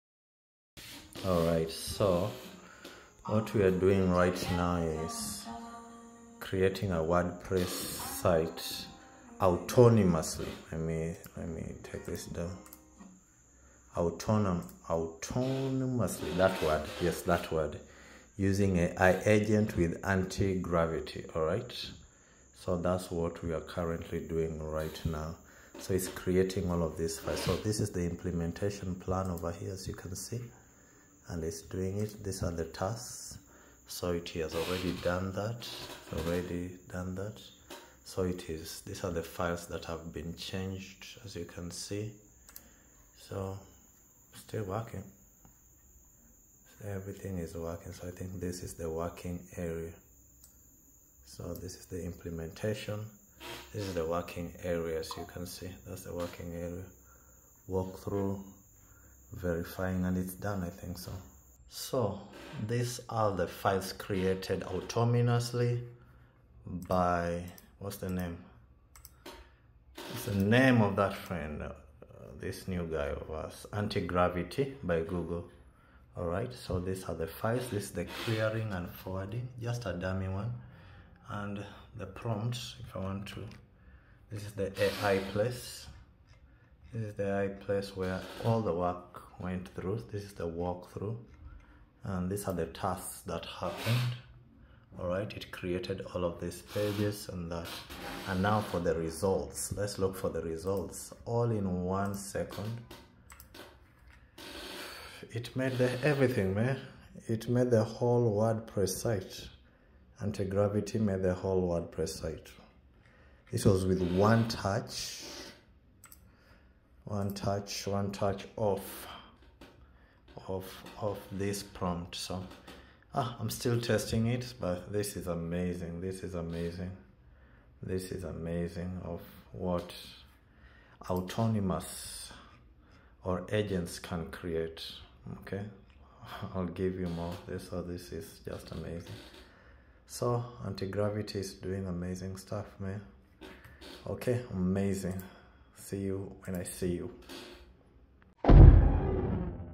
Google Antigravity Built a WordPress Site With One Prompt
I wanted to see how far Google Antigravity has really gone… so I gave it one prompt and told it to build an entire WordPress site from scratch.
No extra guidance. No step-by-step. Just one instruction.
What happened next honestly surprised me.
In this video, you’ll see:
How Antigravity behaves like an autonomous agent

How it handled theme creation, folder structure, CSS, PHP, and templates

How it built a complete homepage inside WordPress

How close we are to true one-prompt website generation

This is not a polished demo , this is a rough test and a first touch, but the results were way cleaner than I expected.
If this is what Google is doing on a rough attempt… you can imagine what’s coming next.